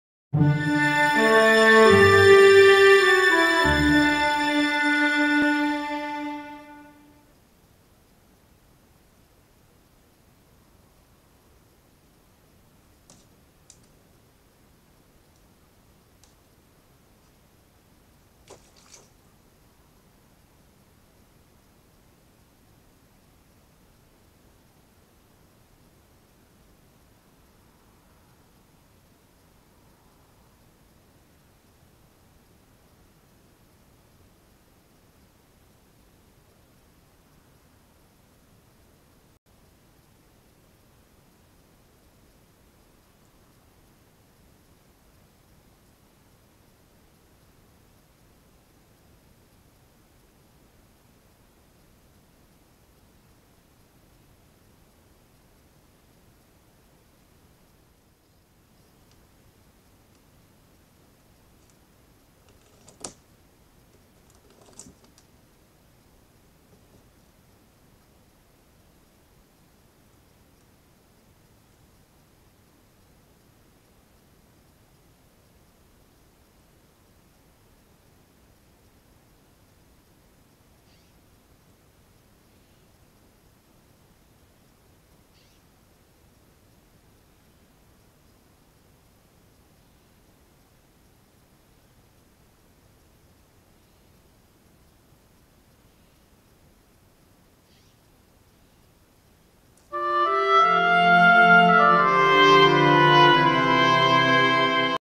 Great horned owl takes bath
A great horned owl takes a bath in the oak woodlands of the Southern Sierra Nevadas.